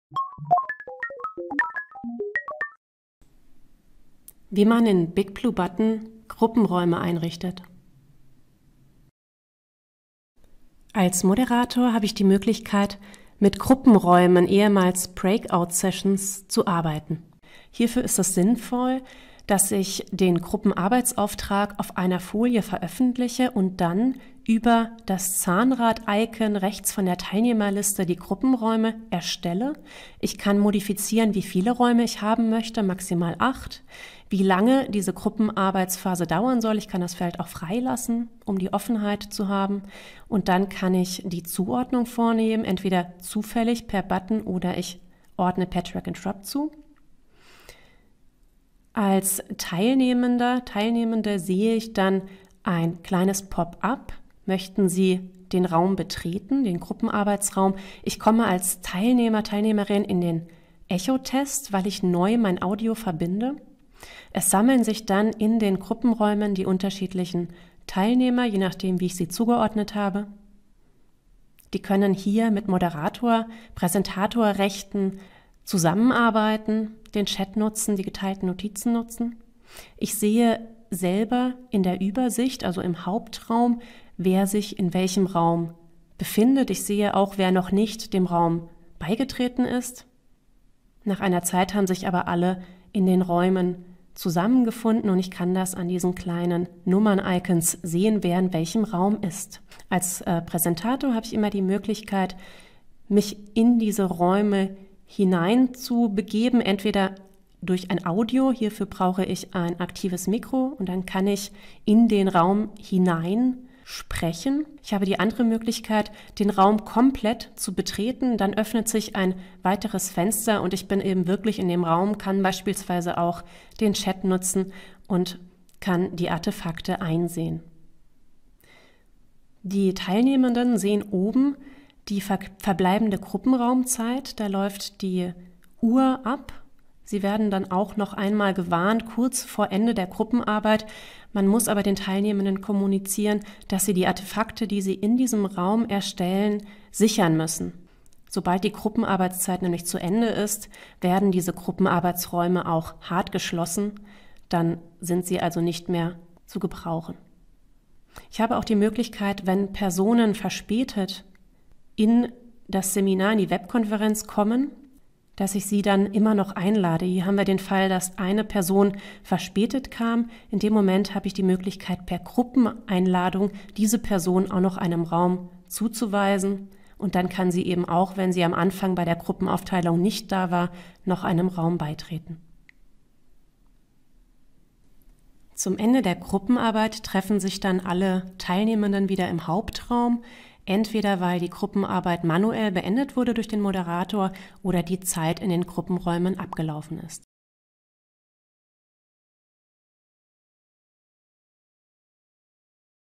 BigBlueButton - Gruppenarbeitsräume einrichten
In BigBlueButton können während einer laufenden Webkonferenz Gruppenarbeitsräume (ehemals Breakout-Räume) eingerichtet werden, um der Zusammenarbeit in Gruppen einen geschützten Raum zu bieten.